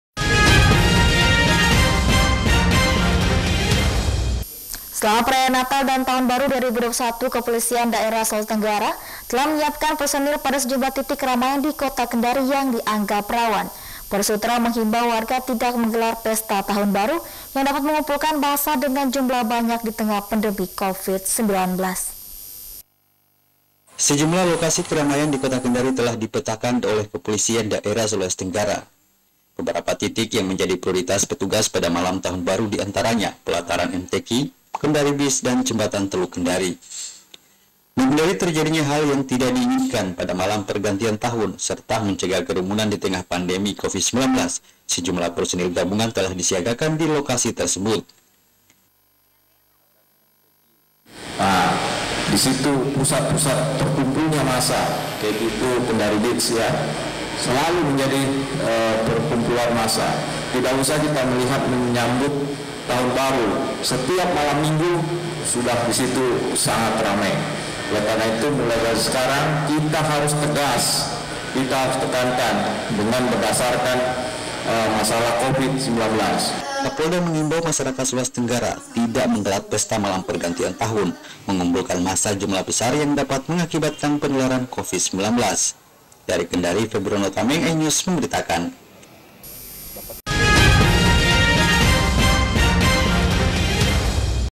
Polda Sultra Antisipasi Kerumunan di Sejumlah Tempat saat Malam Pergantian Tahun
iNews Sultra, 25 Desember 2020